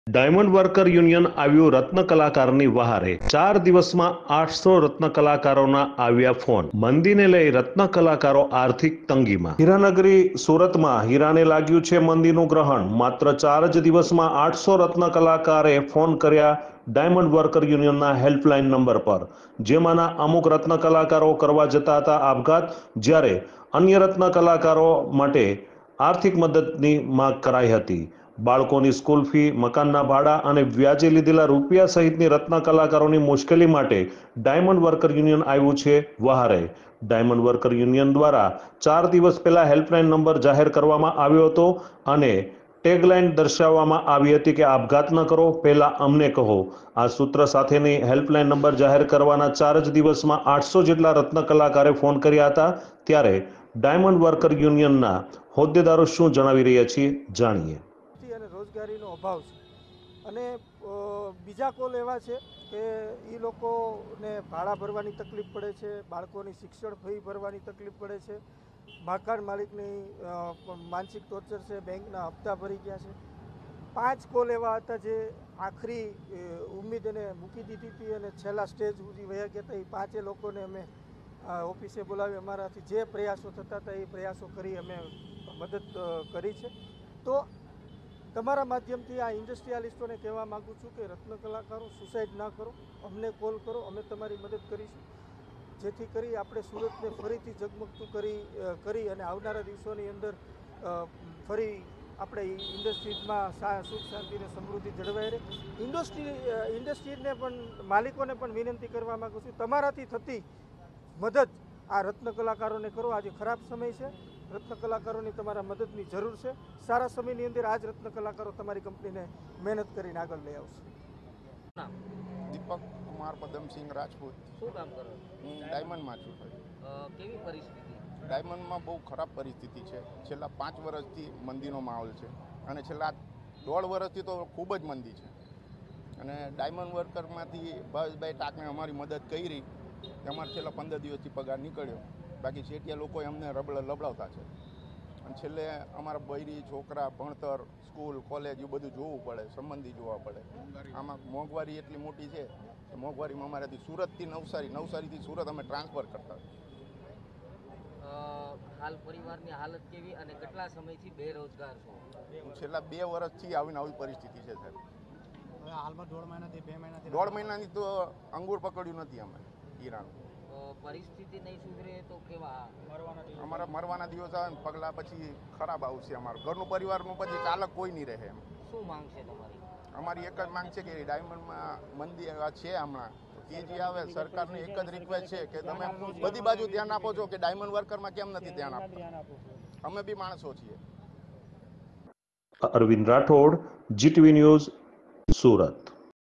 Surat:- ડાયમંડ વર્કર યુનિયન આવ્યું રત્નકલાકાર ની વ્હારે...|| GTV_GUJARAT_NEWS
ડાયમંડ વર્કર યુનિયન આવ્યું રત્નકલાકાર ની વ્હારે